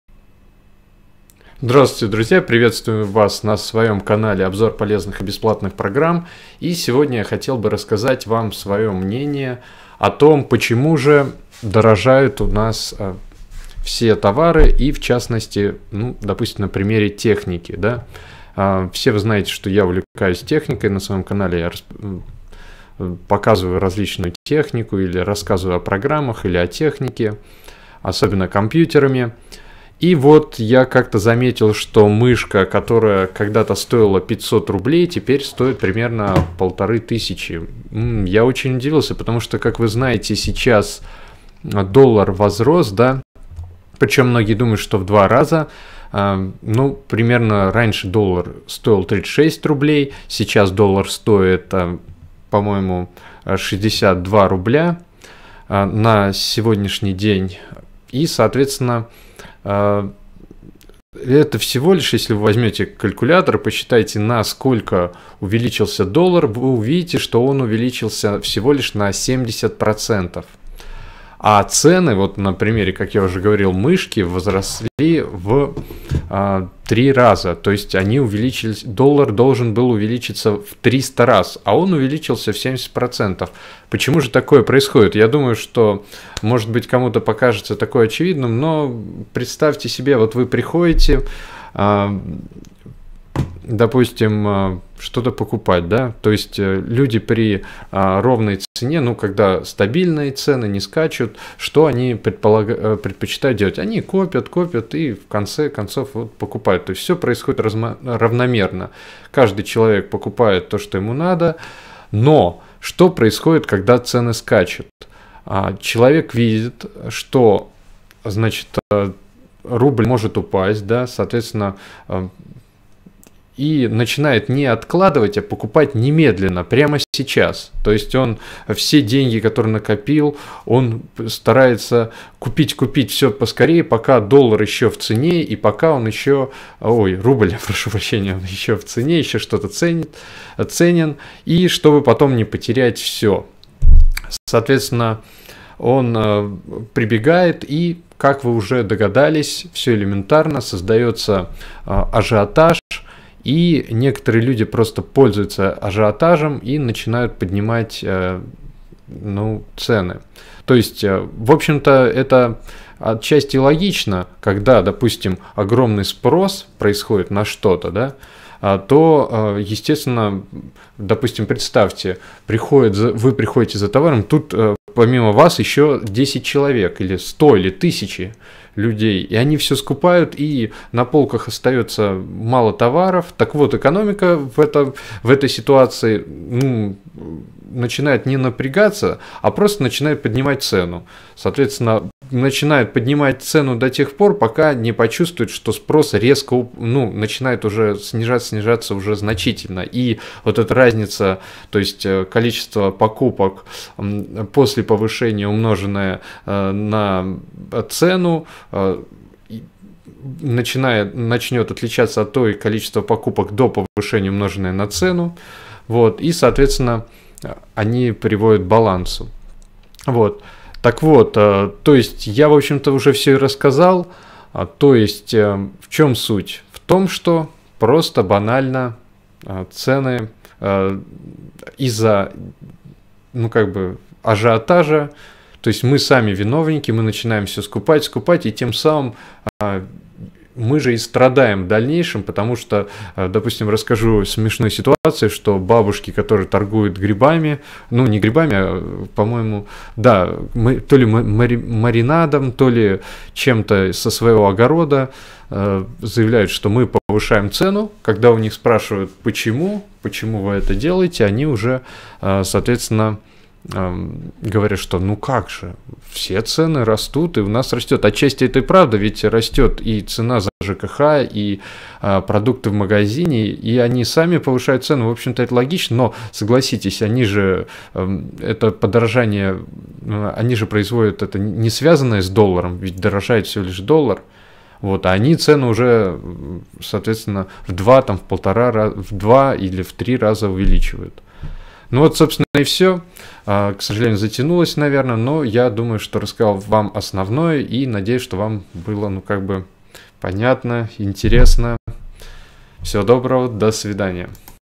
Почему дорожают товары и продукты в 3 раза
Доллар вырос на 76 % , а цены возросли почти на 300 %